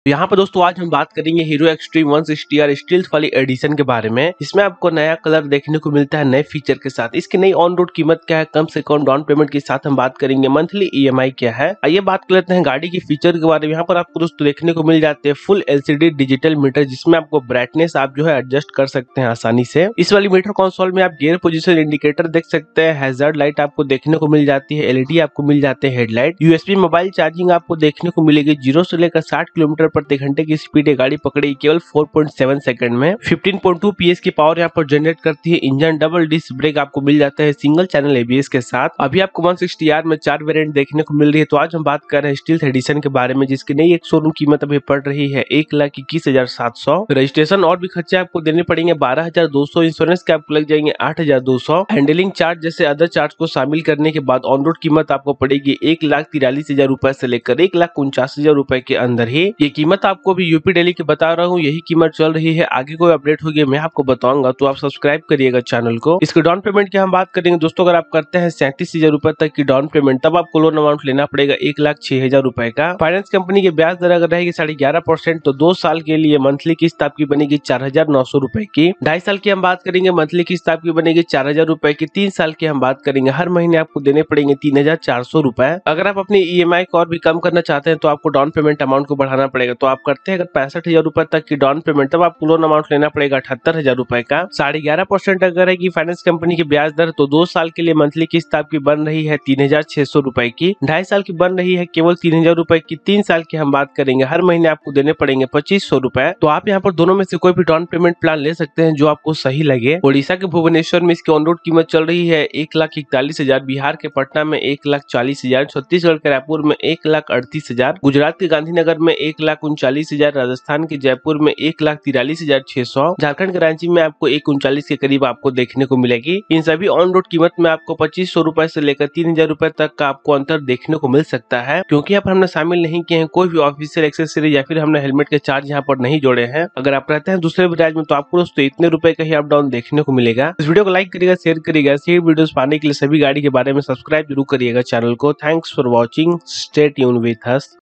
2022 Hero Xtreme 160R Stealth Edition Price | Xtreme 160R Stealth 2022 on road price,downpayment,emi
Hero Xtreme 160R Stealth Edition New 2022 model On road price. Explained BS6 New colour Hero Xtreme 160R Stealth Edition downpayment and monthly emi details.

before buy hero xtreme 160r jane Hero Xtreme 160R Stealth Edition cash price & finance details